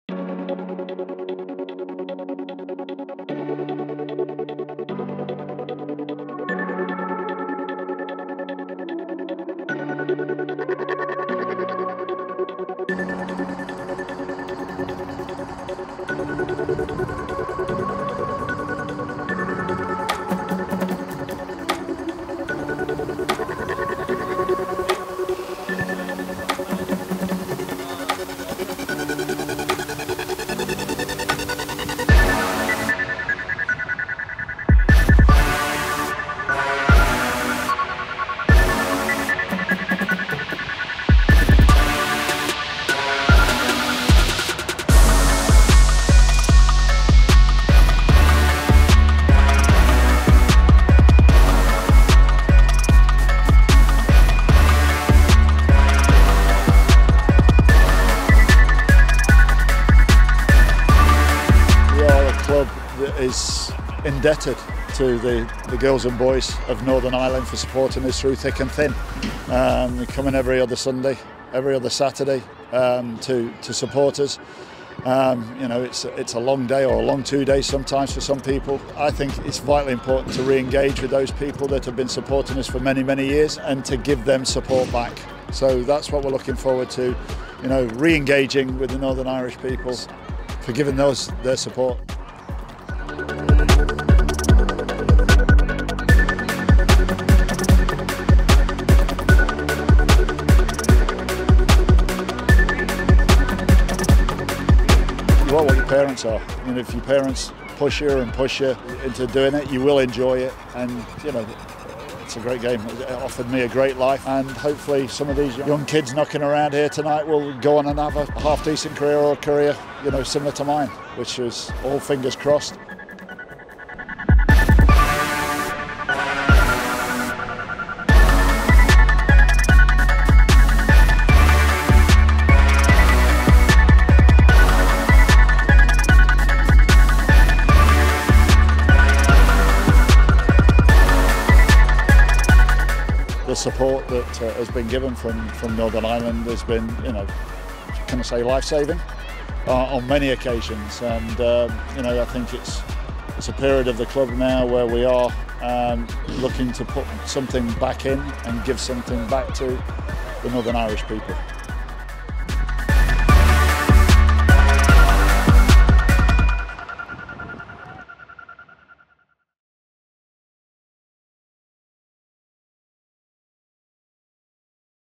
SOCCER ACADEMY | Northern Ireland Camp | 2024
Catch some of the action from Rangers Soccer Academies in Northern Ireland.